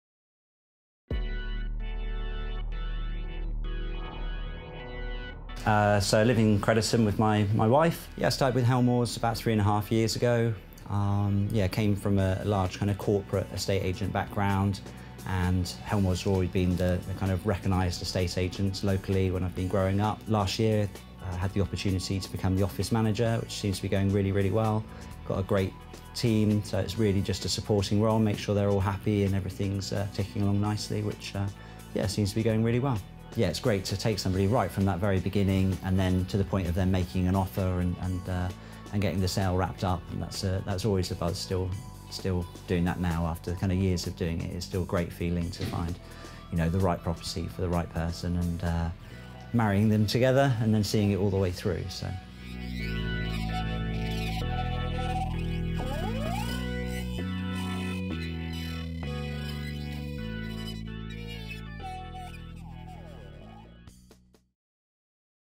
Interview with Ian @ Helmores
An interview with Ian, a member of staff at Helmores in Crediton.